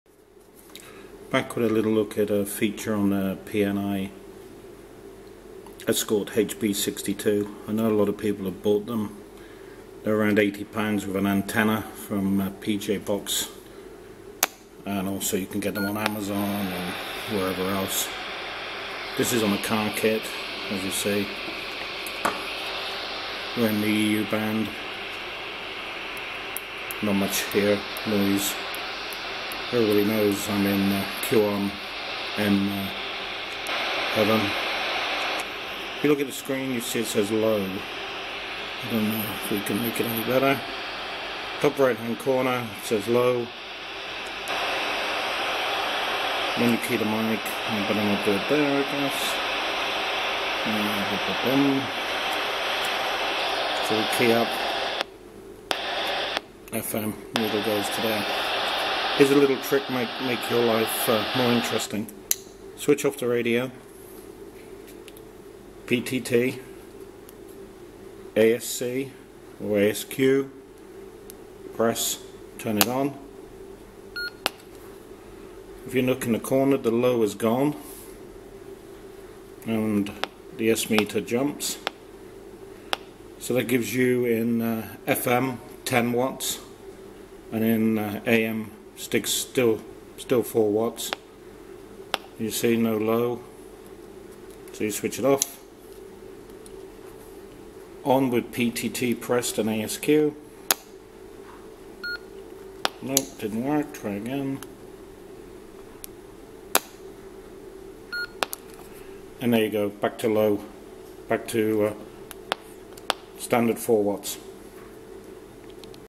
PNI ESCORT HP 62 (Hidden Extra's) Part 1
I know a lot of people have bought this little radio as my email shows me. Around £80 with an antenna and a car kit so runs 12v from cigarette lighter. The name HP would make you think HIGH POWER? Well if it does we will show you below how to activate 4w AM (So it does not change) and extend the FM power to 10 watts. Maybe interesting for some people?